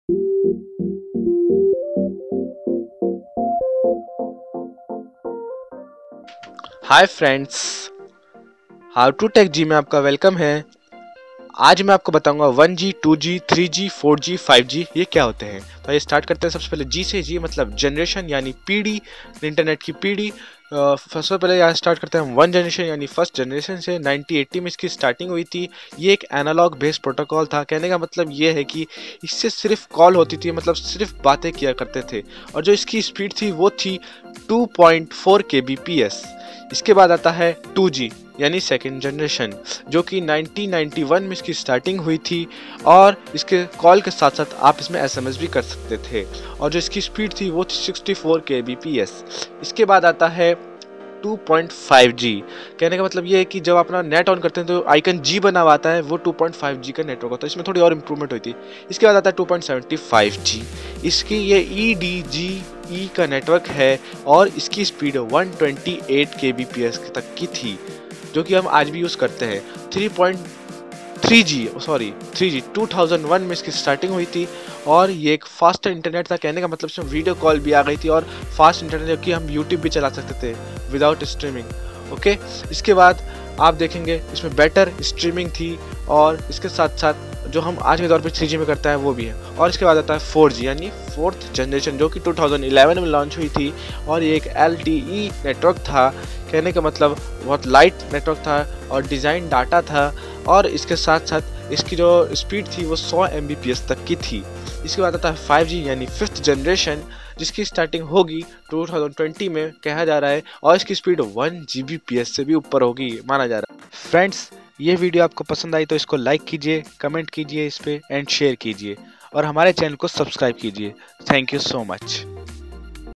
What is 1g 2g 3g 4g 5g network in hindi
Hi Friends

iss video main me aapko btaonga ki 1G 2G 3G 4G 5G network yeh kya hote hai.